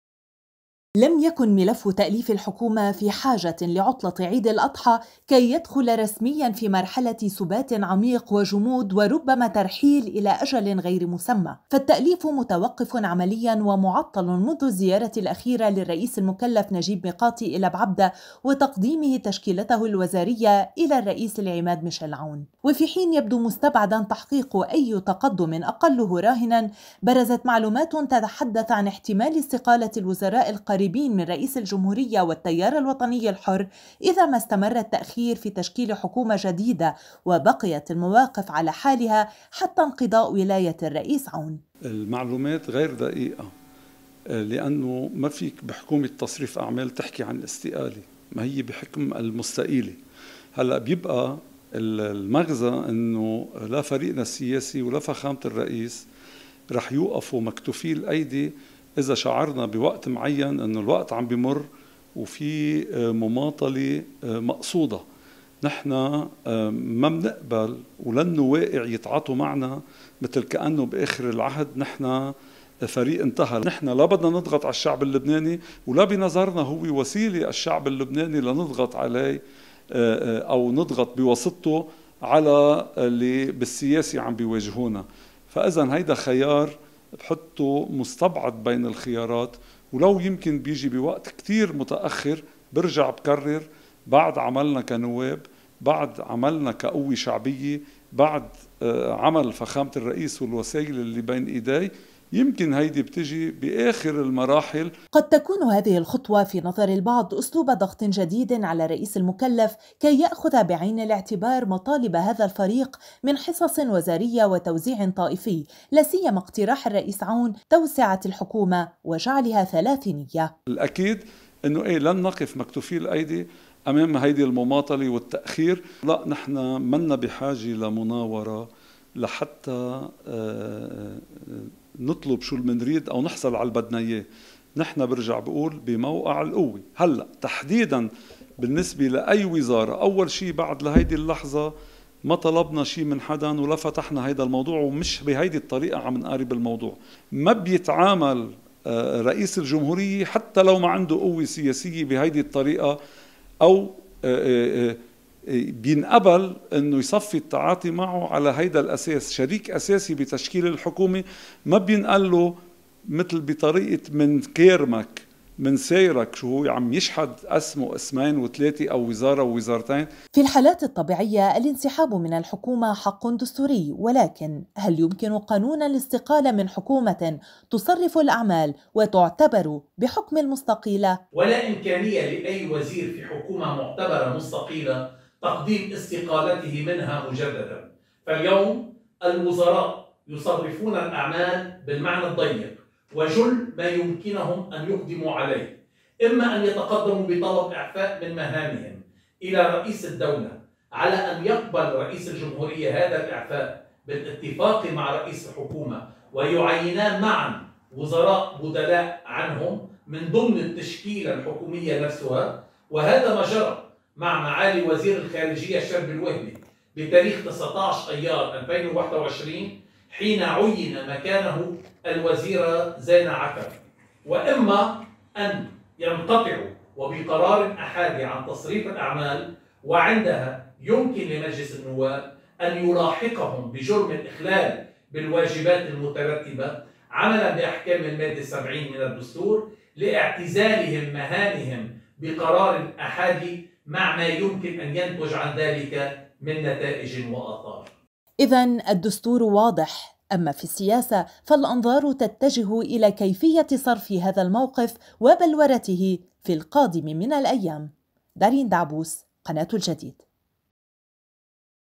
الوزراء المقربون من التيار "لن يستقيلوا".. والتوقف عن تصريف الأعمال "آخر الحلول"!
هل يستقيل الوزراء القريبين من رئيس الجمهورية والتيار الوطني الحر ردا على التأخير في تأليف حكومة جديدة؟ وهل من استقالة من الاستقالة؟ وما مفعولها القانوني؟